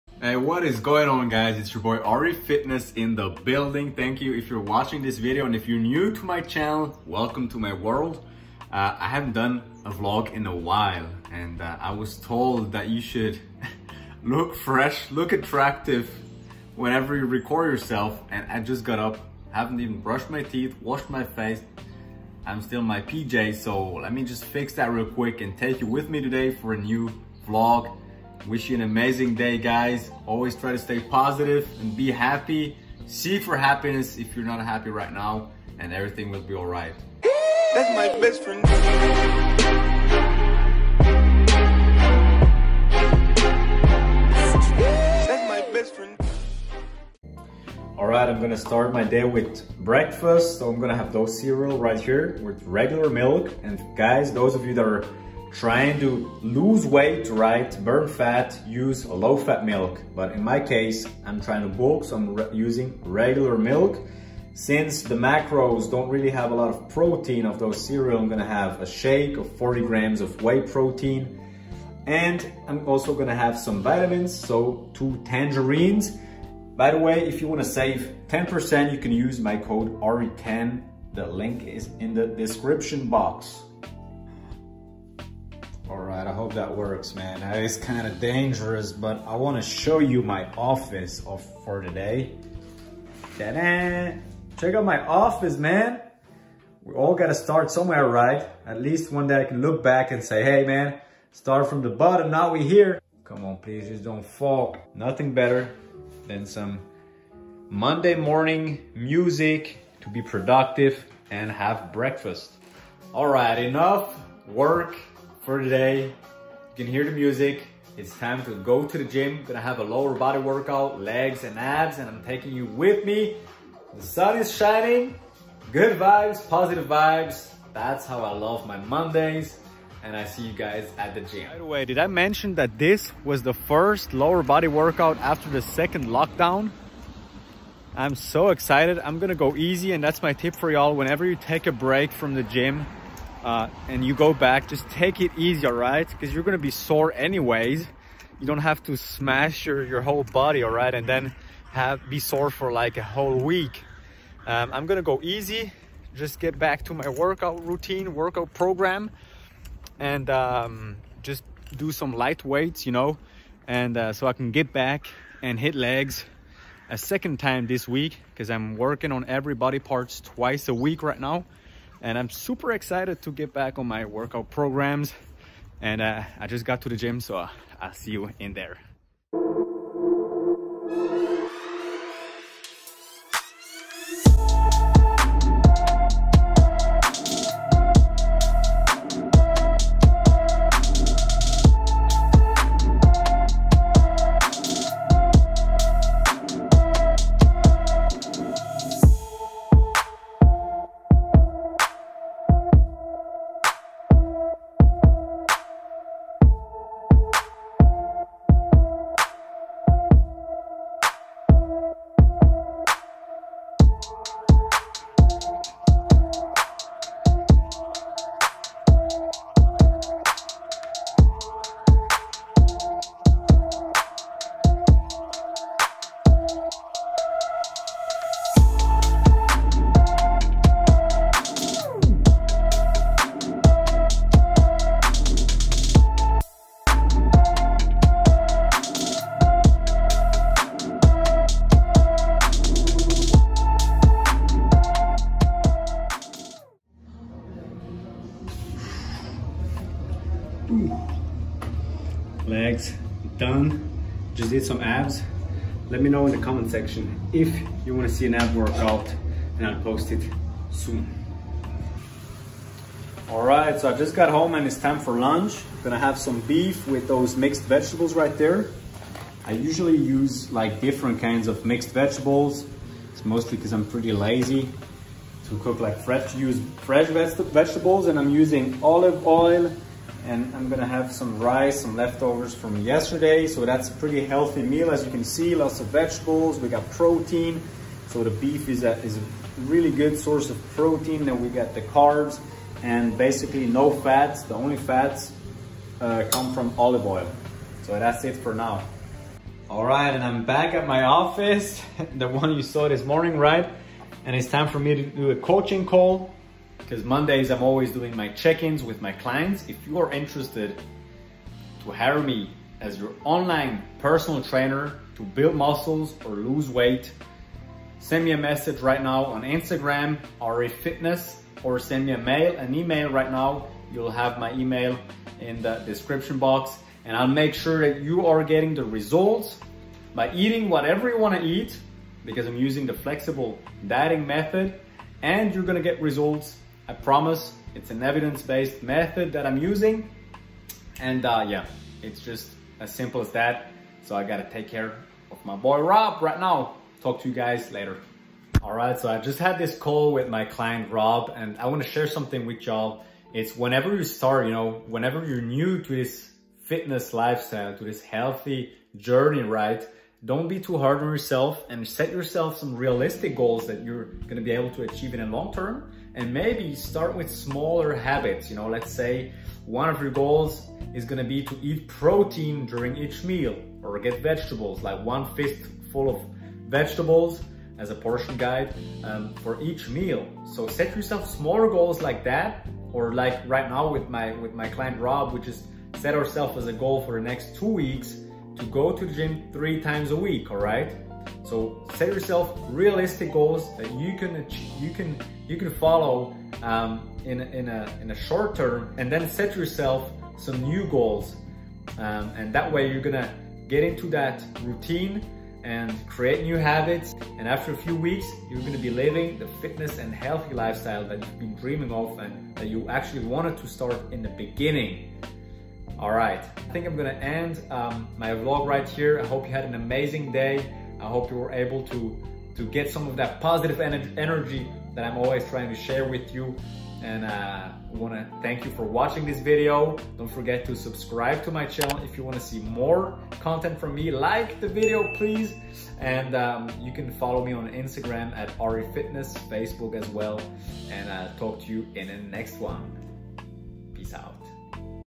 Best Tip if you're New to the Fitness Lifestyle | YOUR ONLINE COACH | Comeback VLOG
Hey! My name is Auri and I certified as a Fitness Trainer because I'm passionate about helping people during their fitness journey, especially when it comes to building muscles and gaining weight.

In this comeback vlog I tried to pass some positive energy and fitness motivation your way and share MY TIP NR1 IF YOU'RE NEW TO THIS FITNESS & HEALTHY LIFESTYLE: Set small but realistic goals that you will be able to expand later on but make sure you create a habit first.

You can hire me as your Online Personal Trainer.

Talk to you soon, your online coach Auri